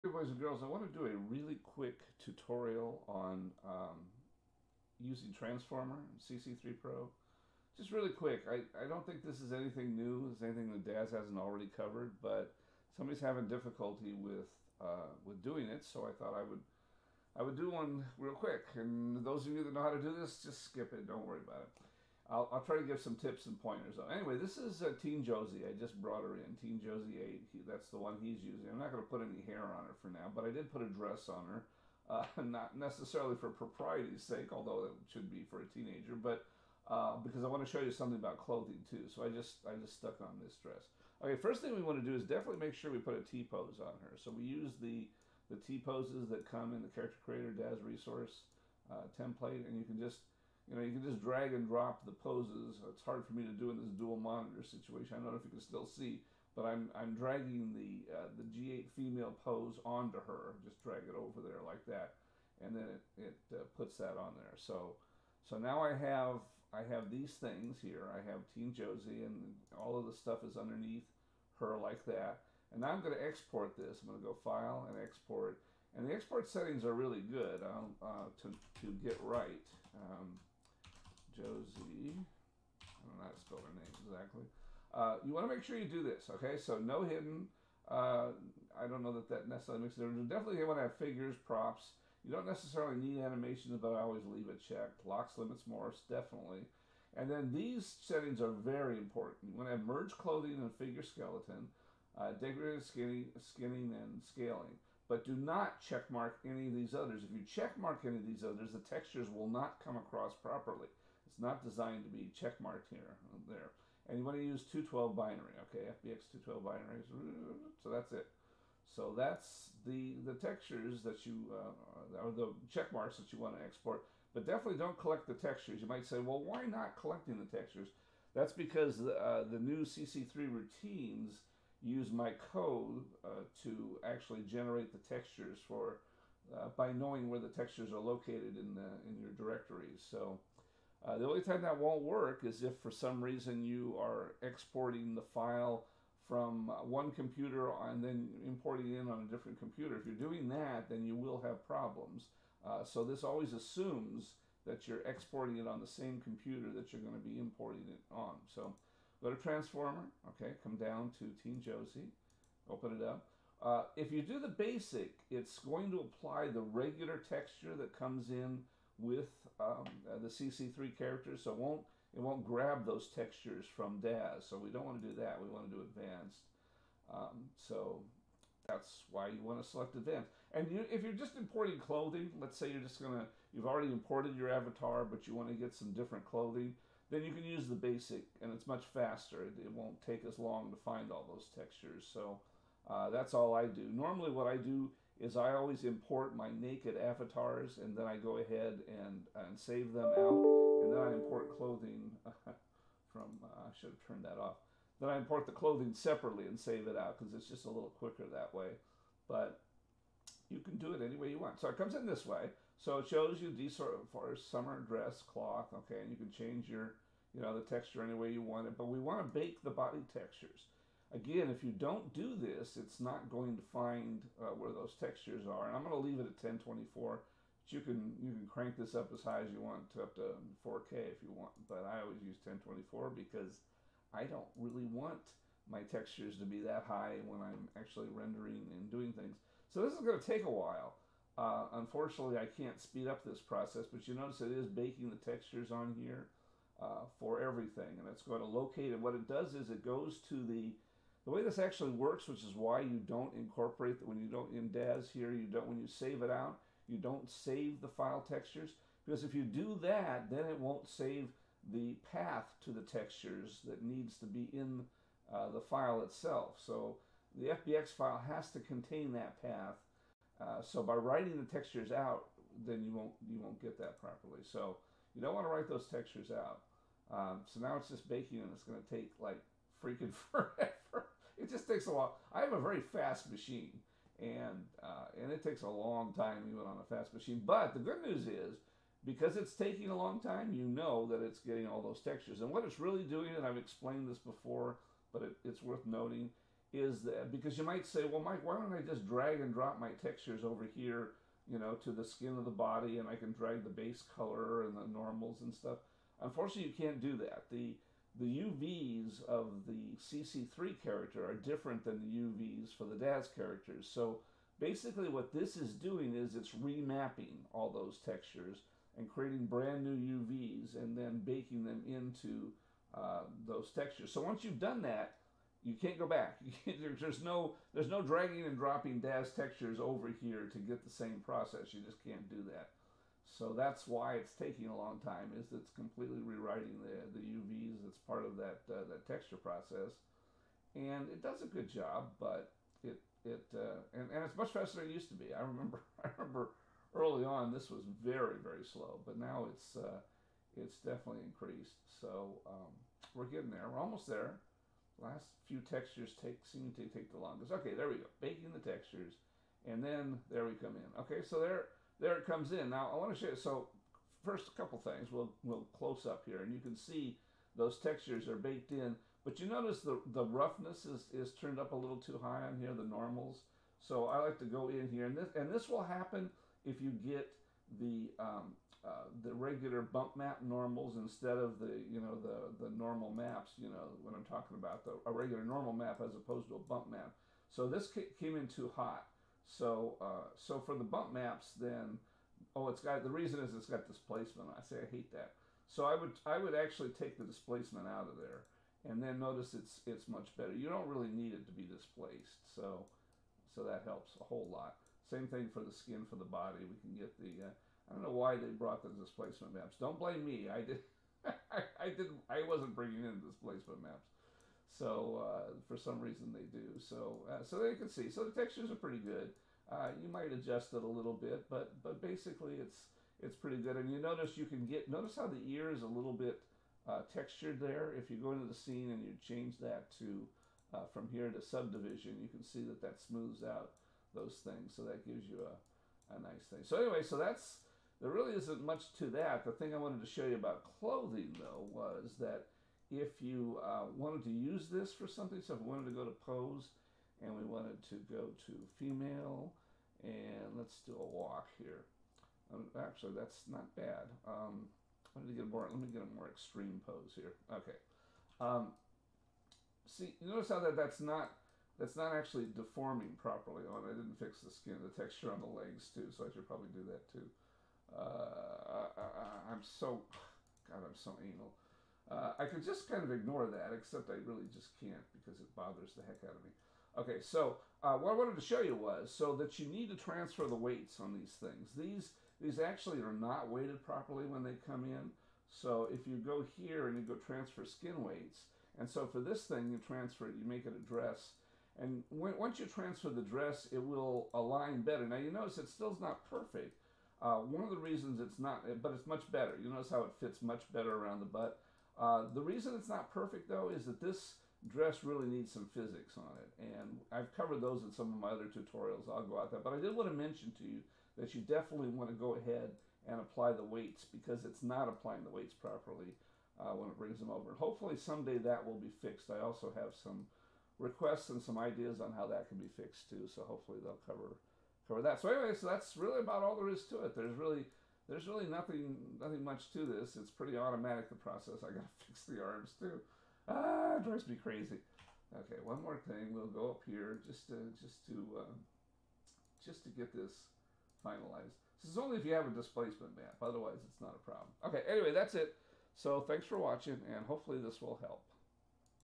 Down and dirty using Transformer in CC3
A VERY quick run through of how to use Transformer in CC3 to bring Daz avatars and clothing in (there's nothing much to it but someone on the forum asked, and I am a sucker for helping).